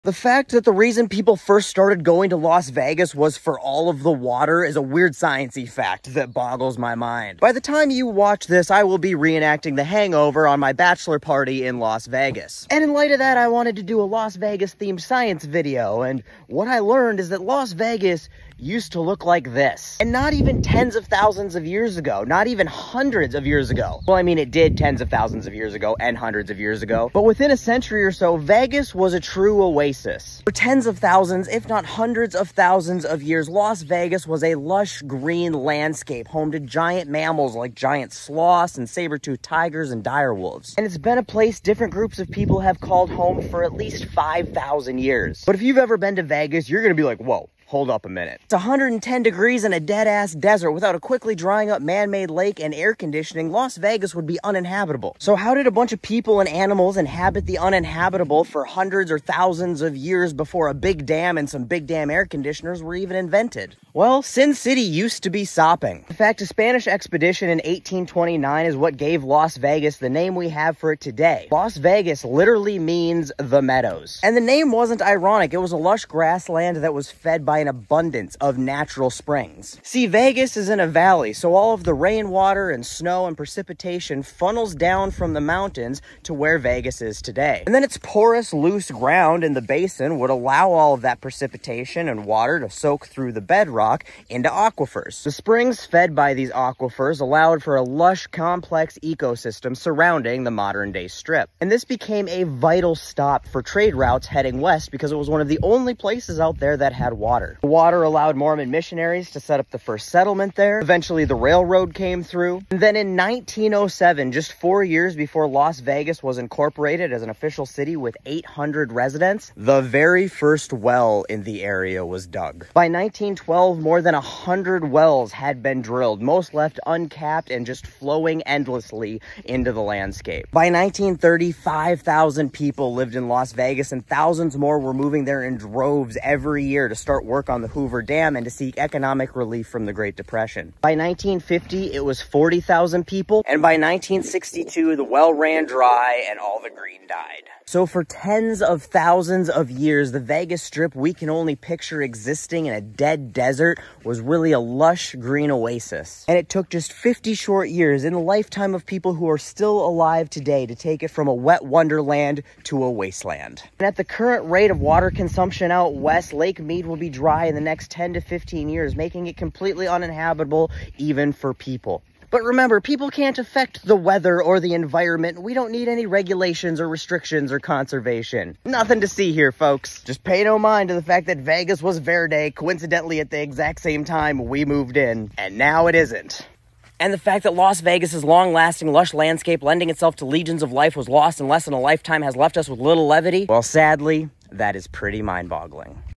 Science! Las Vegas was Verde
science! Las vegas was once a lush oasis not long ago.